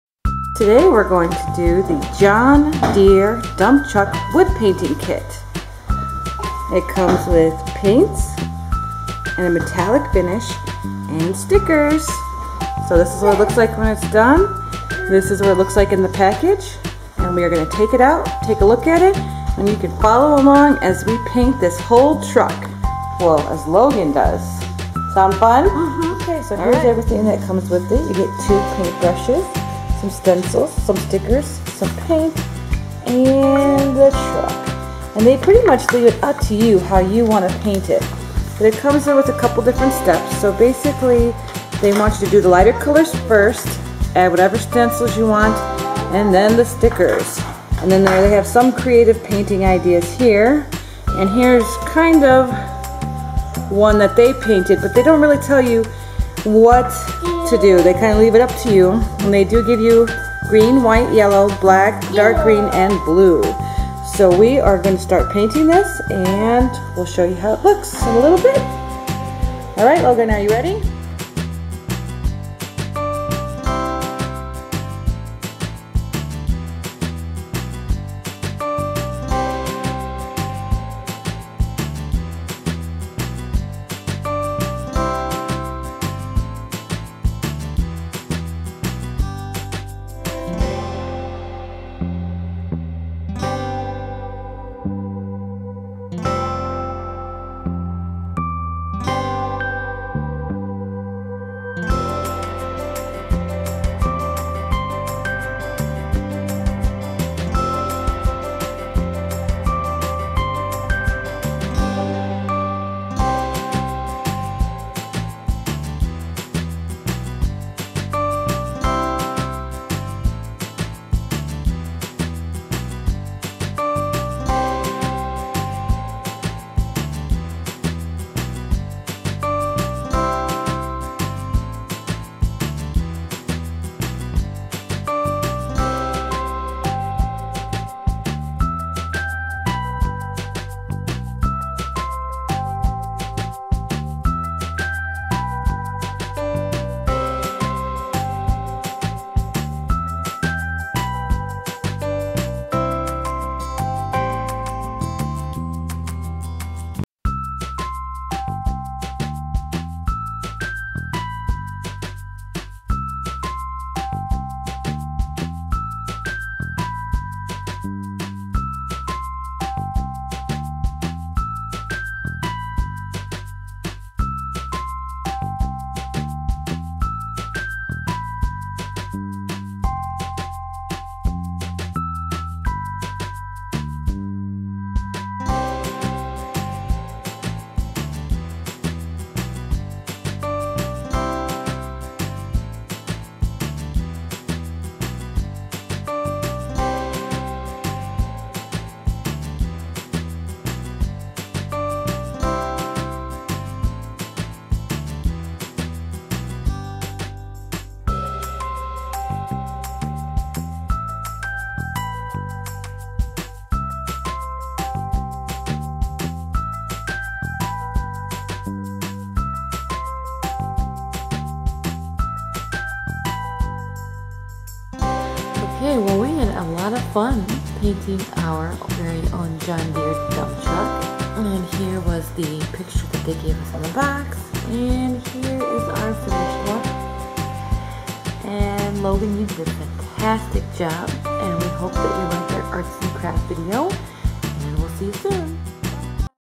DIY John Deere Dump Truck Wood Painting Kit!
Today we are doing a fun art & crafts set! What could be more fun than panting your very own John Deer Dump Truck!! Follow along with Logan from beginning to end as we show your the wooden dump truck fresh out of the box all the way to the end! Complete with paint, stickers and fun!!